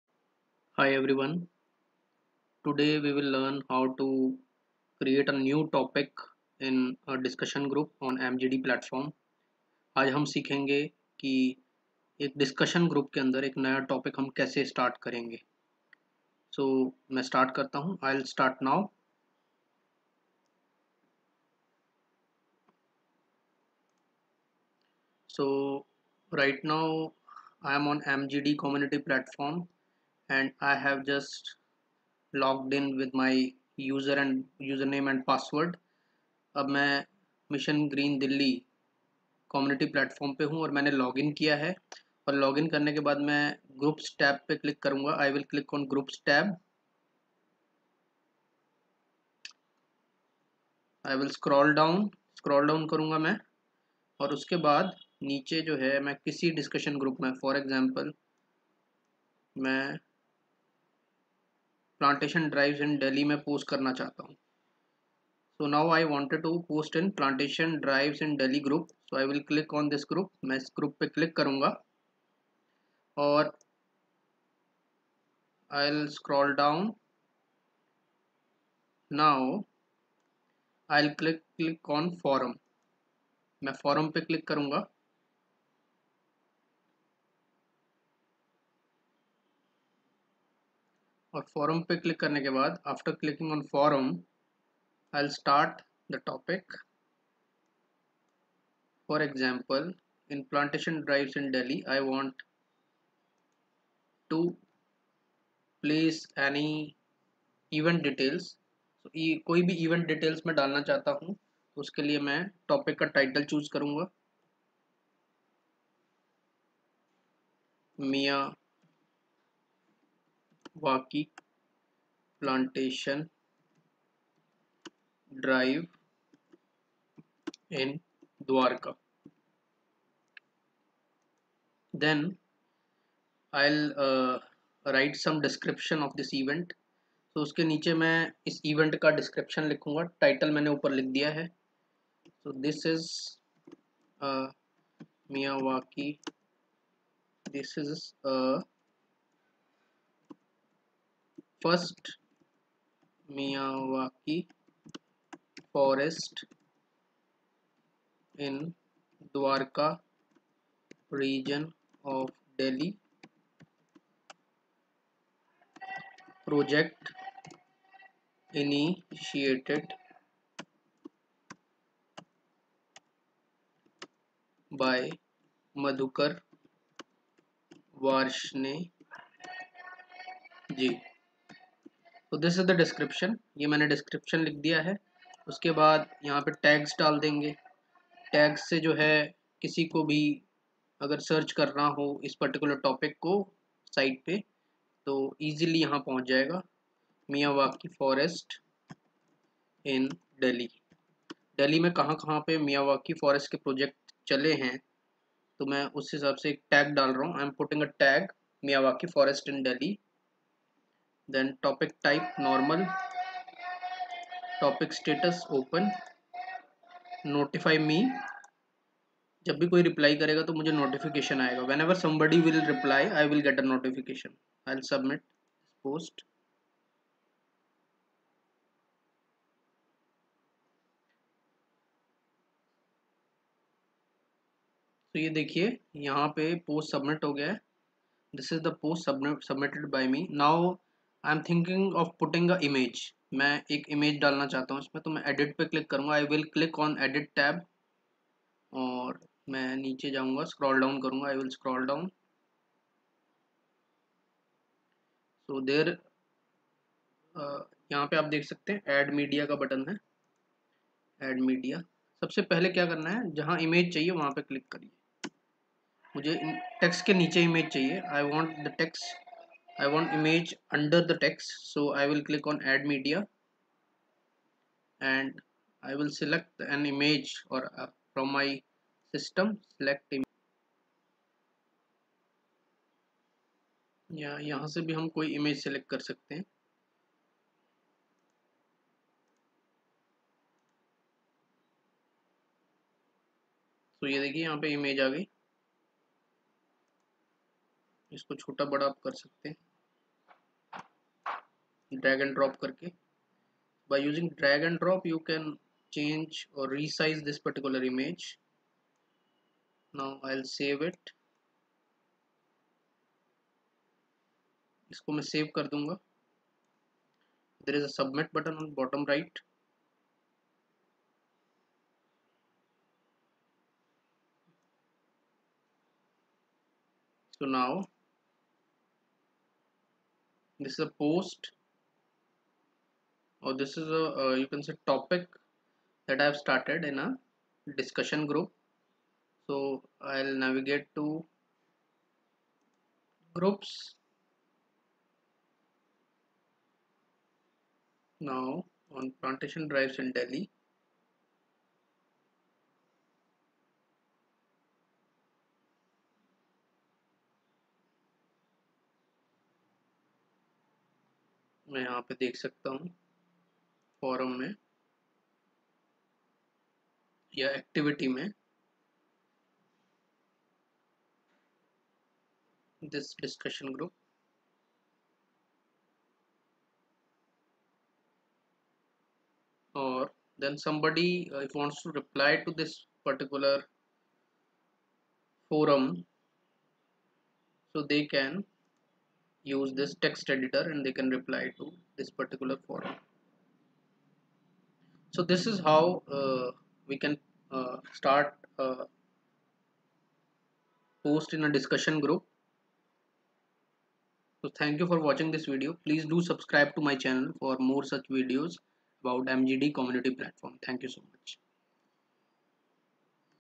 Starting new topic in discussion group | MGD Community Platform | Session 002
This training session demonstrates how to start new forum topics in discussion groups on MGD Community platform. to register.